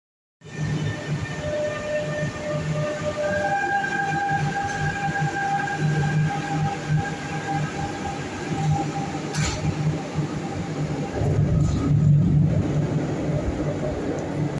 Subte Línea C: Formación CNR CITIC Equipo (B) (202) Saliendo De La Estación Avenida De Mayo.
En este vídeo vemos a la Formación CNR CITIC Equipo (B) (202) de la Línea C de Subtes de Emova saliendo de la estación Avenida De Mayo rumbo a Constitución.
Composición: Rca202, Mpa202/Mpb202, M202 y Rc202.

Filmado el día Miércoles 16/11/2022 a las 16:16 hs.